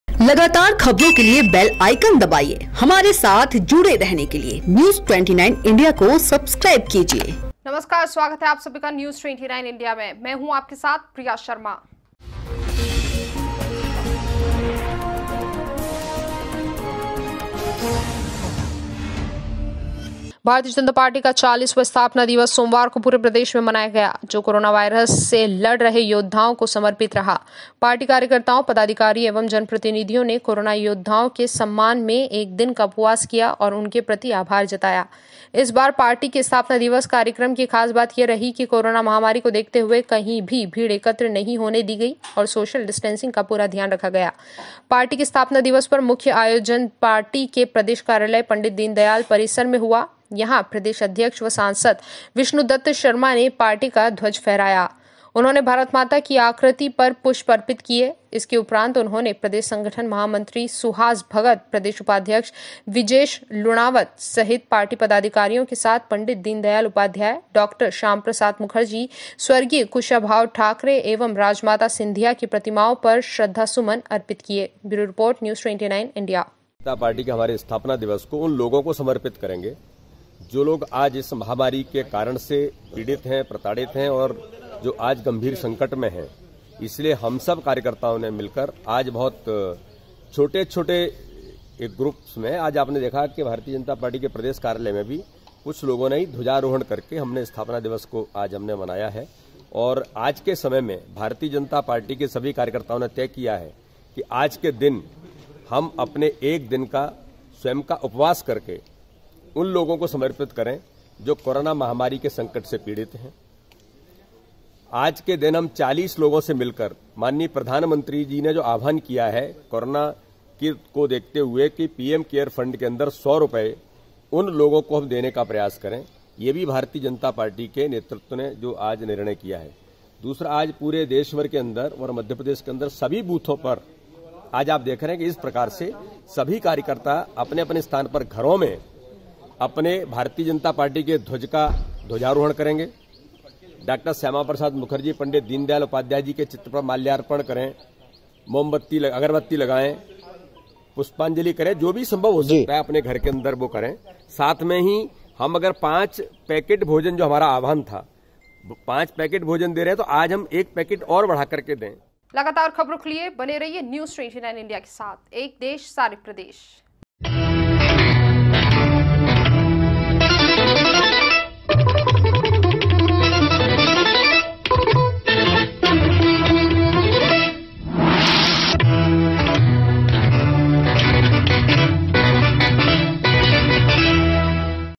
06_April News29India# News 34#कोरोना योद्धाओं का आभार जताकर भाजपा ने मनाया स्थापना दिवस
कोरोना योद्धाओं का आभार जताकर भाजपा ने मनाया स्थापना दिवस
कोरोना को देखते हुए कहीं भी भीड एकत्र नहीं होने दी